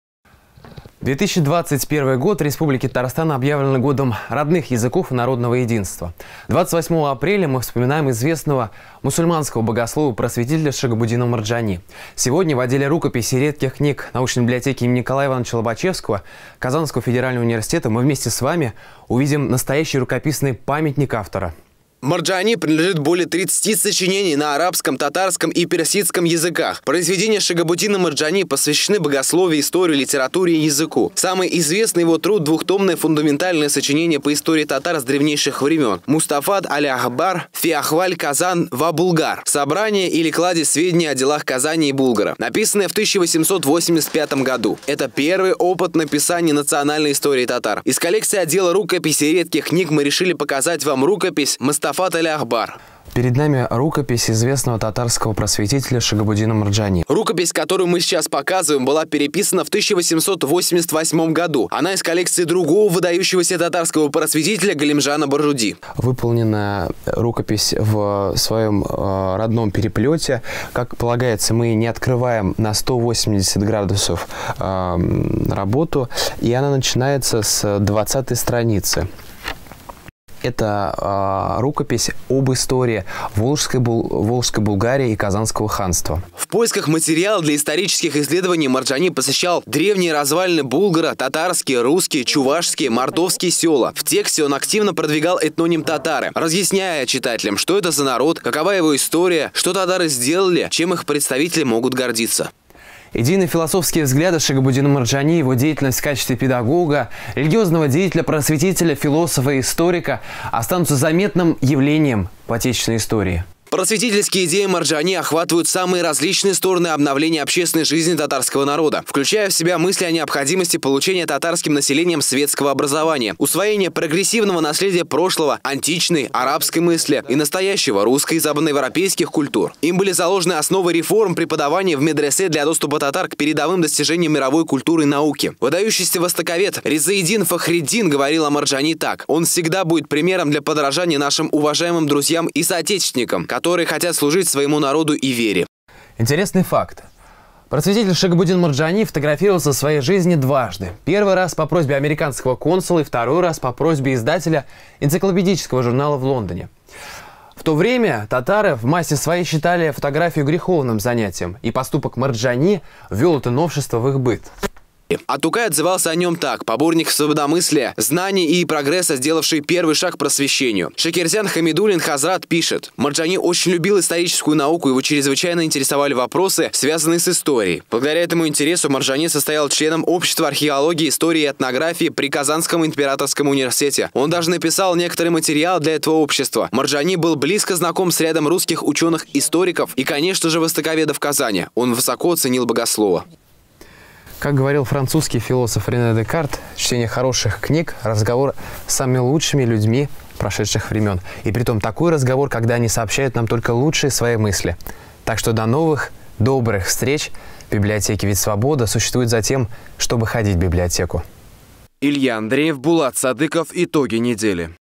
Литературное наследие КФУ: Рукописный памятник Шигабутдина Марджани
В фонде библиотеки Казанского федерального университета хранятся произведения татарского ученого-мыслителя, религиозного и общественного деятеля Шигабутдина Марджани.